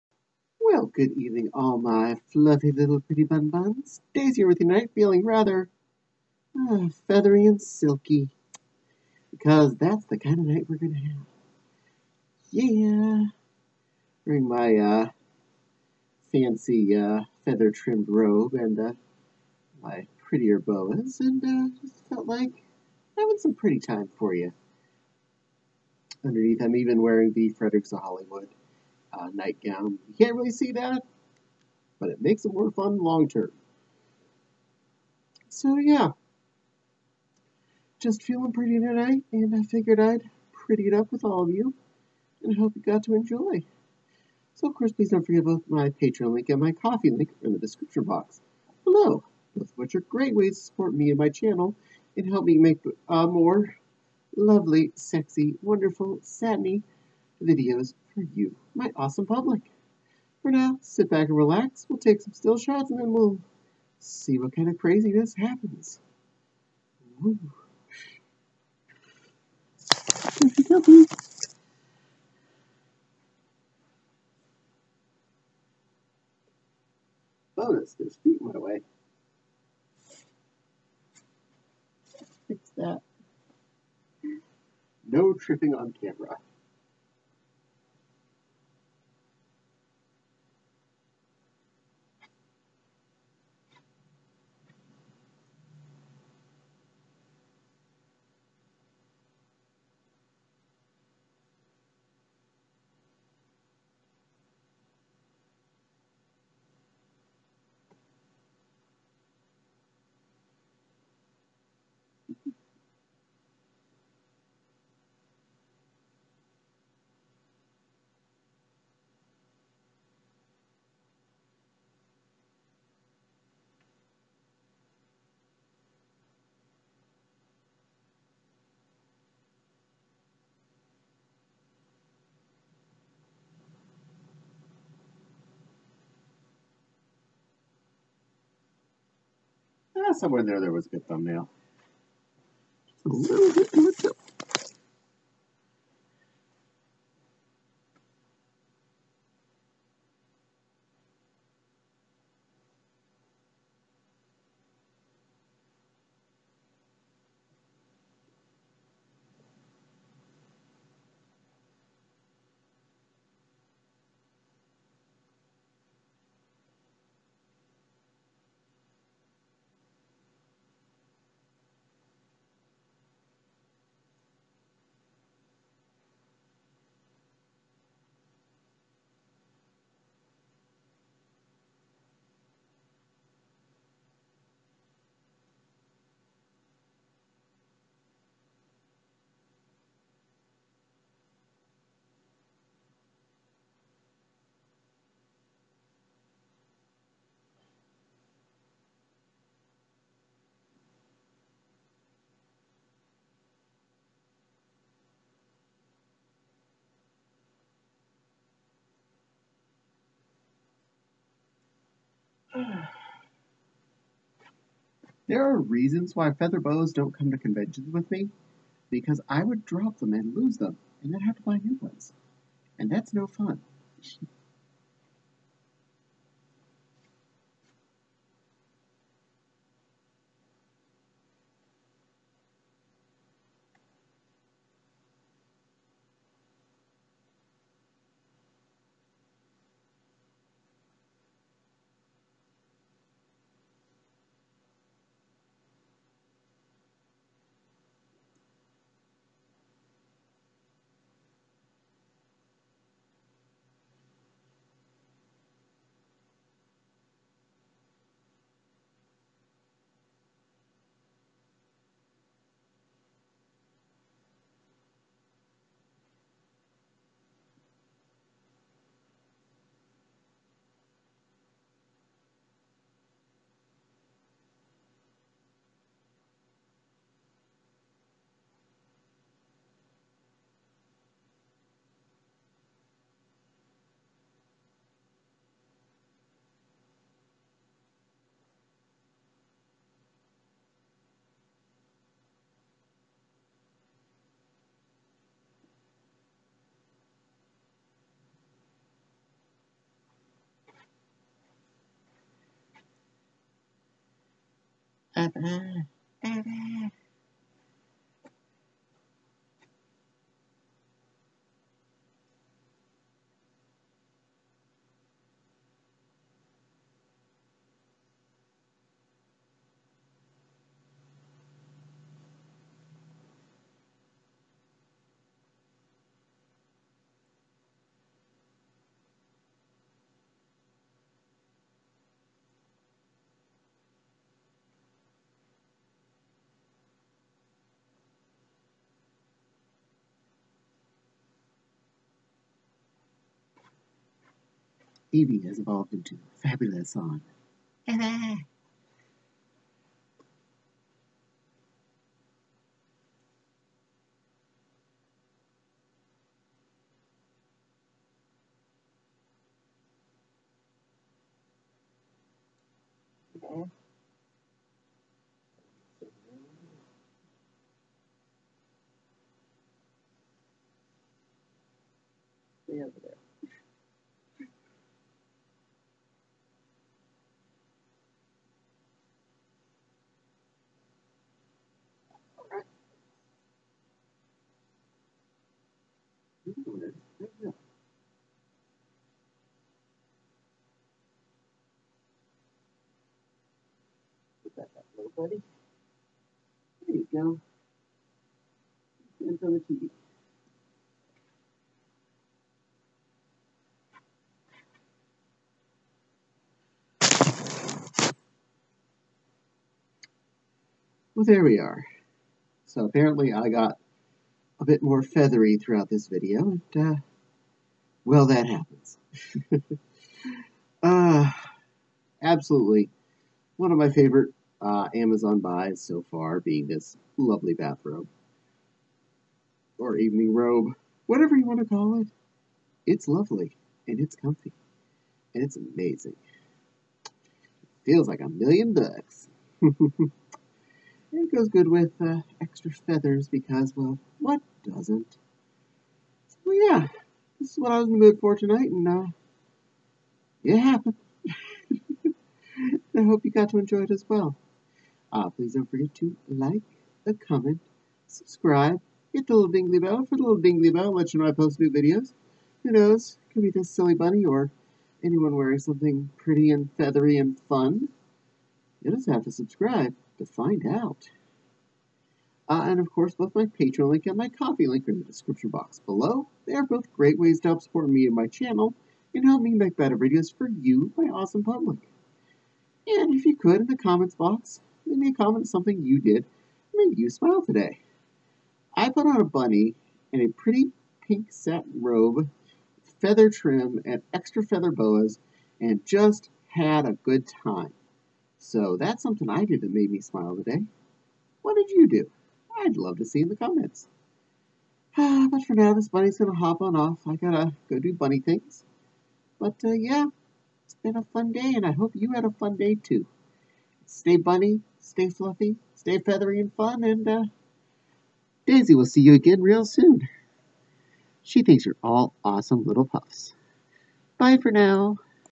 Daisy Sexy Satin and Feathers Full suit video
Daisy having a comfy time in my satin robe with feather trim again!